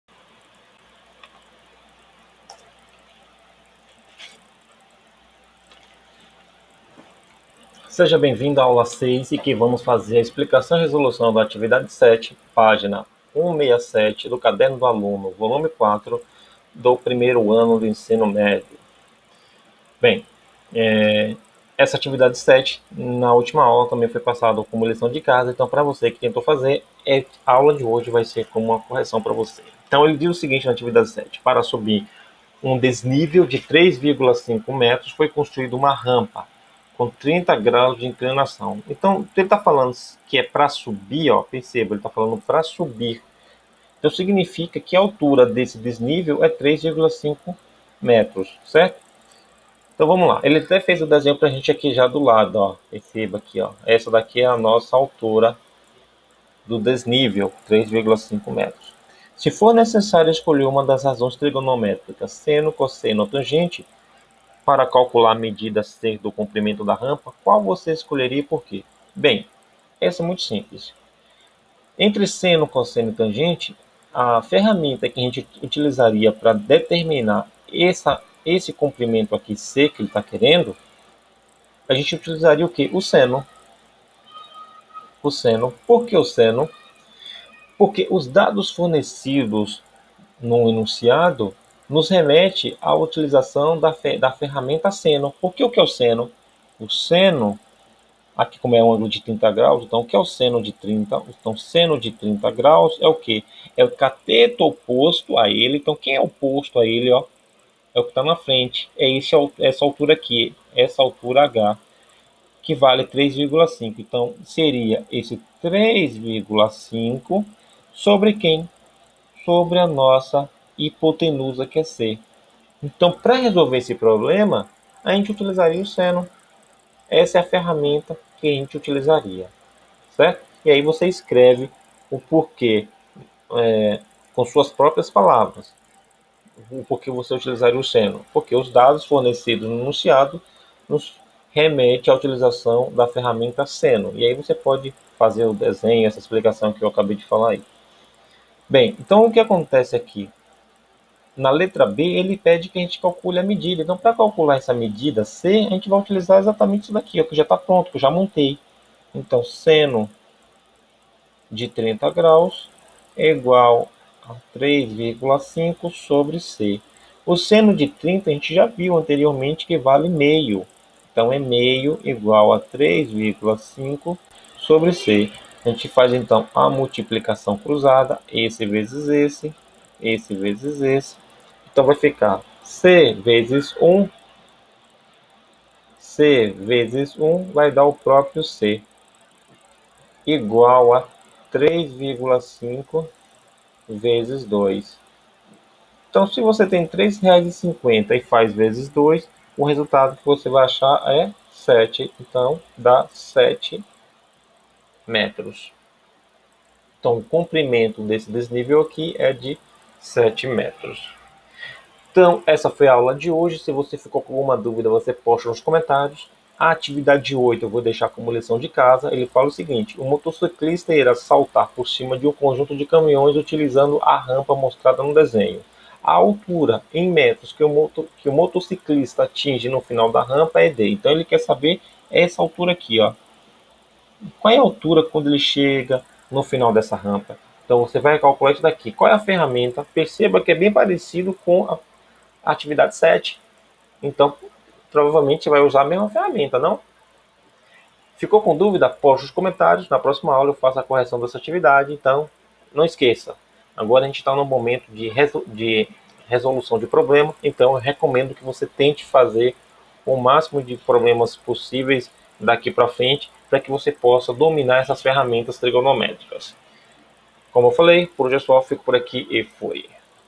Tema 1: Razões Trigonométricas no Triângulo Retângulo - Aula 6
Essa é a aula 6, tema 1 do quarto bimestre do primeiro ano do ensino médio.

Justine Classroom